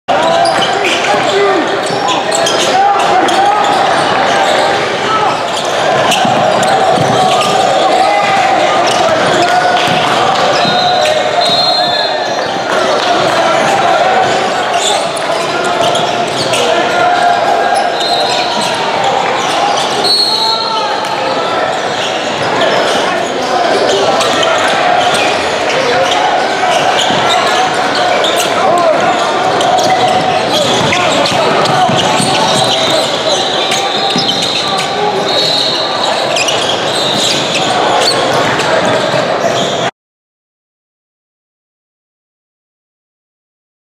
Video highlights: Class of 2025 Melvin Bell vs. Squadron at 2021 CP3 Rising Stars Camp
Video highlights of class of 2025 wing Melvin Bell of Chicago (Ill.) St. Rita vs. Squadron at 2021 CP3 Rising Stars Camp on Aug. 21, 2021, in Greensboro, N.C.